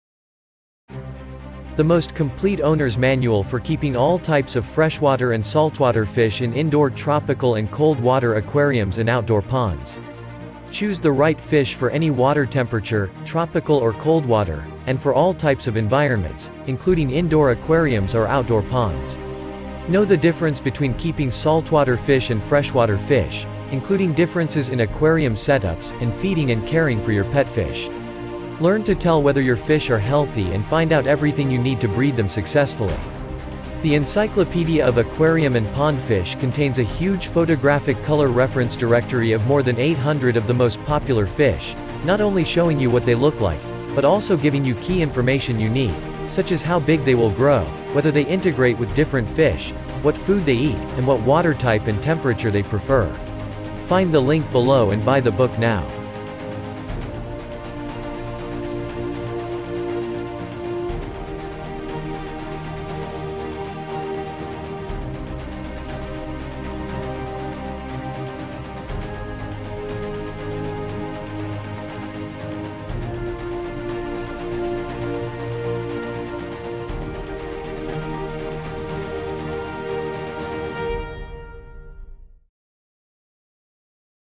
Encyclopedia of Aquarium and Pond Fish
The most complete owner's manual for keeping all types of freshwater and saltwater fish in indoor tropical and coldwater aquariums and outdoor ponds.

Choose the right fish for any water temperature--tropical or coldwater--and for all types of environments, including indoor aquariums or outdoor ponds. Know the difference between keeping saltwater fish and freshwater fish, including differences in aquarium setups, and feeding and caring for your pet fish. Learn to tell whether your fish are healthy and find out everything you need to breed them successfully. The Encyclopedia of Aquarium and Pond Fish contains a huge photographic color reference directory of more than 800 of the most popular fish, not only showing you what they look like, but also giving you key information you need, such as how big they will grow, whether they integrate with different fish, what food they eat, and what water type and temperature they prefer.